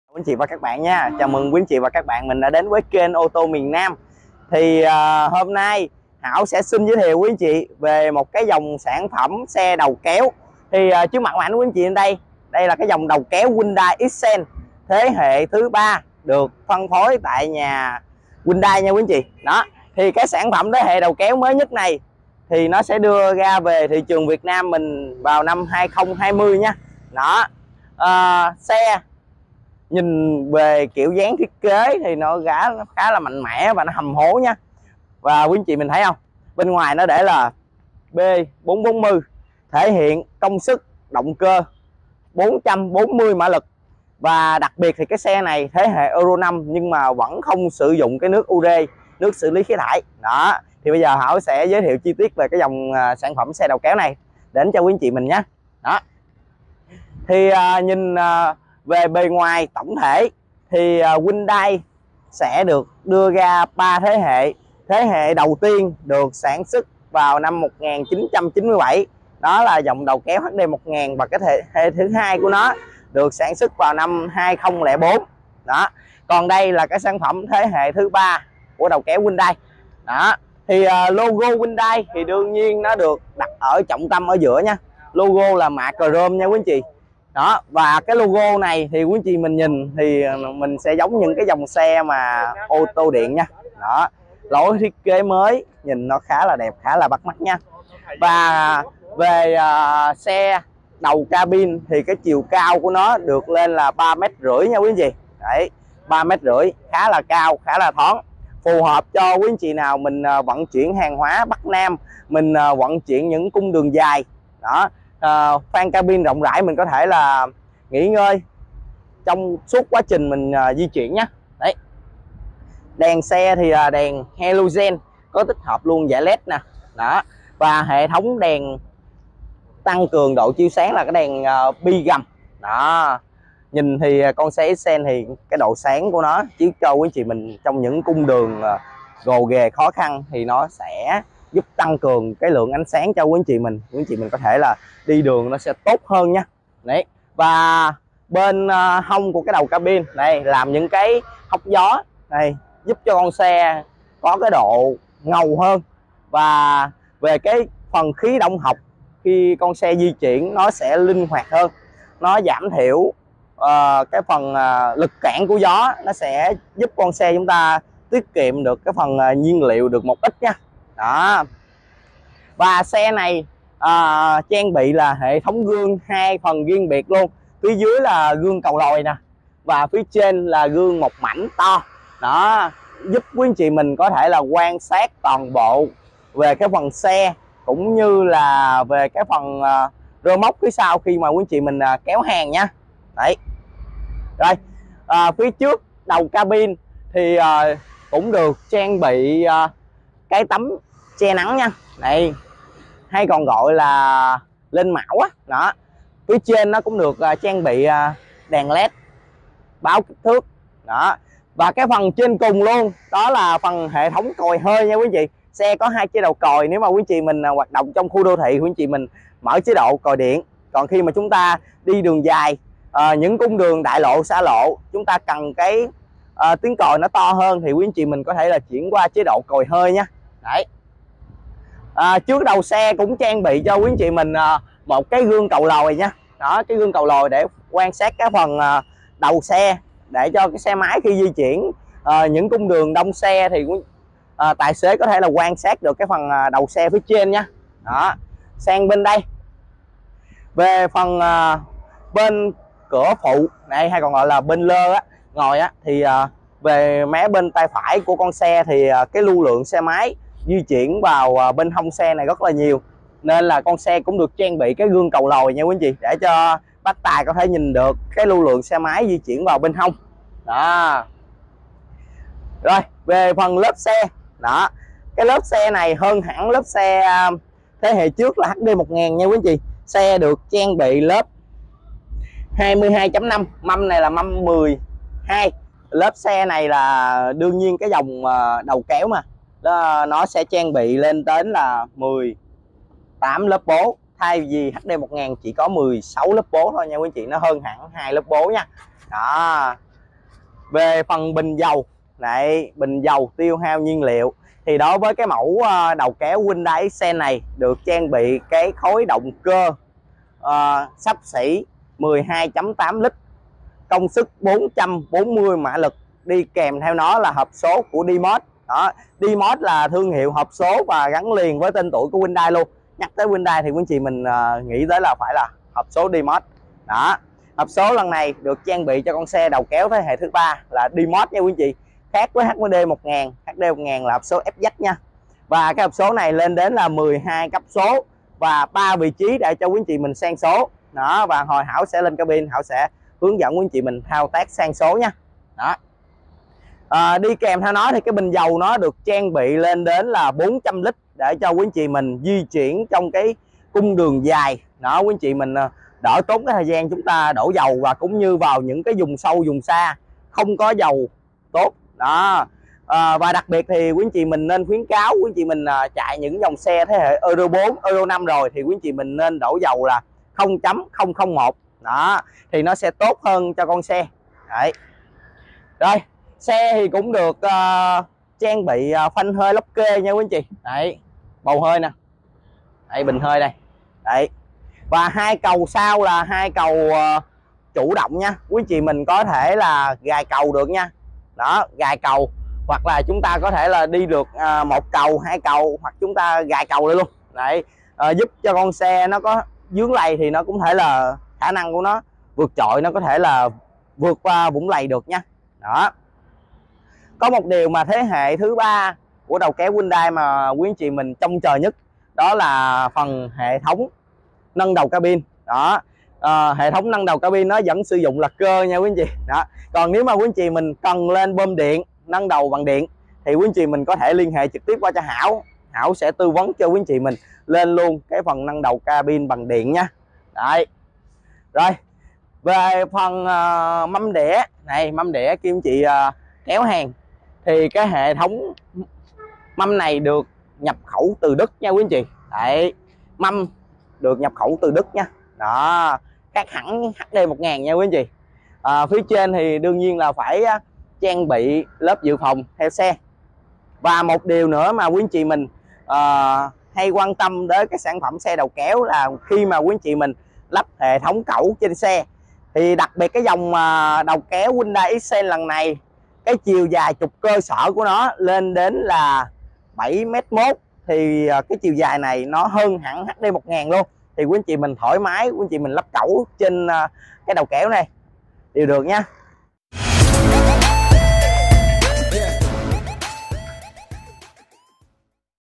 Xe đầu kéo Hyundai thế hệ mới 2022 Xcient 440PS |TỔNG CÔNG TY Ô TÔ MIỀN NAM
Xe đầu kéo Hyundai Xcient GT – Tải trọng lớn, hiệu suất cao, sức mạnh vượt trội.
 Cụm đèn pha và đèn sương mù cỡ lớn mang phong cách mạnh mẽ và hiện đại
Gương chiếu hậu mang phong cách Châu Âu giúp dễ dàng quan sát khi vận hành
 Nội thất tiện nghi, chú trọng mang đến cảm giác thoải mái, thân thuộc cho người dùng, giúp việc lái xe trở nên an toàn hơn.
Cabin có độ bền cao, cấu trúc dạng khung sử dụng thép cường độ cao cải thiện độ an toàn cho người lái khi va chạm.
Tổng Công Ty Ô Tô Miền Nam